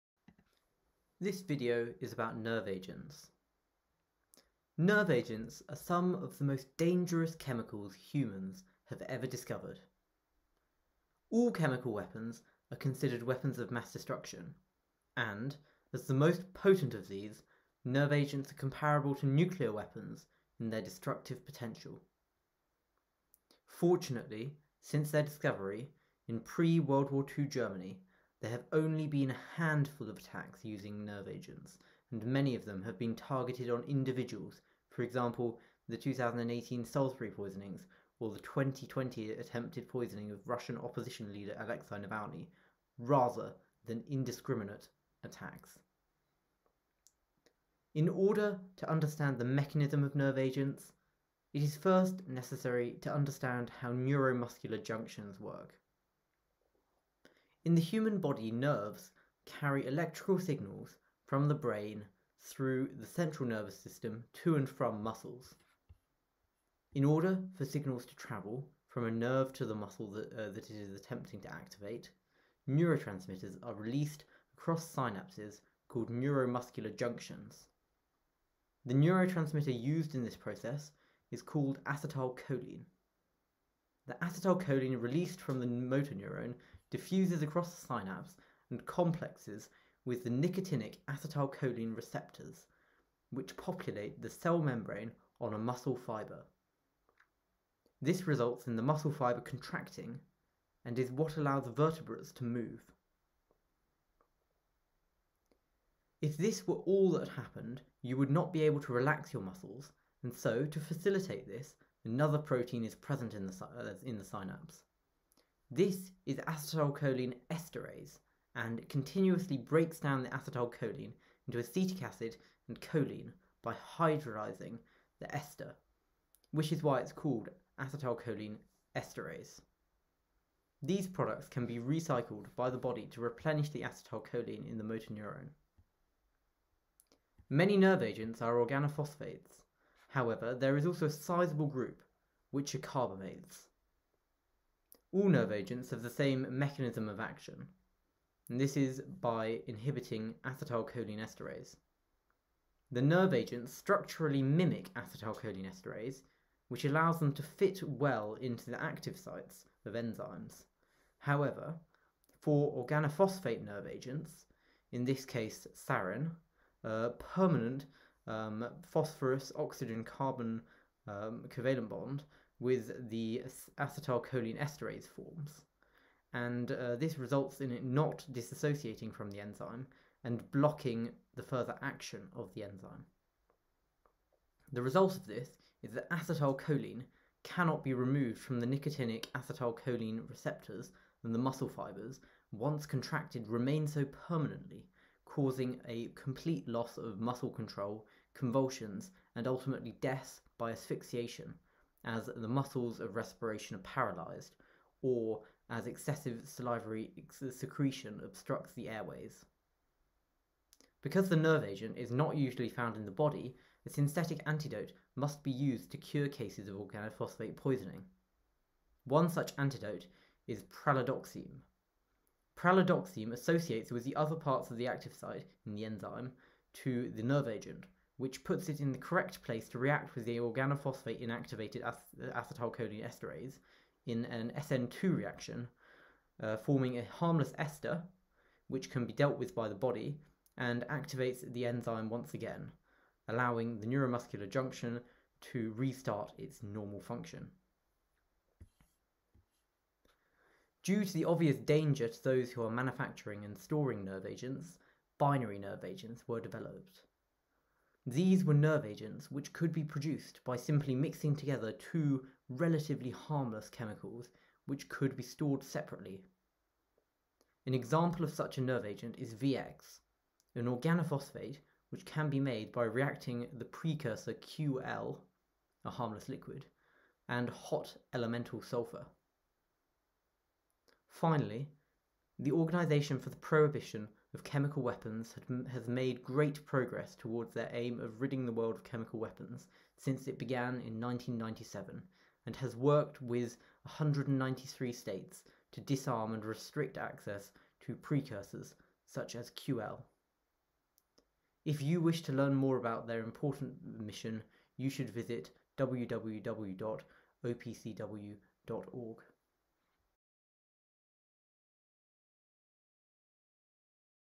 Organophosphate Poisoning and Nerve Agents
for more information about their mission.

The crystalography images were created in PyMOL with data using codes: 2ACE (acetylcholinesterase with acetylcholine), 5FPQ (sarin-inhibited-acetylcholinesterase) and 1UX2 (nicotinic acetylcholine receptor).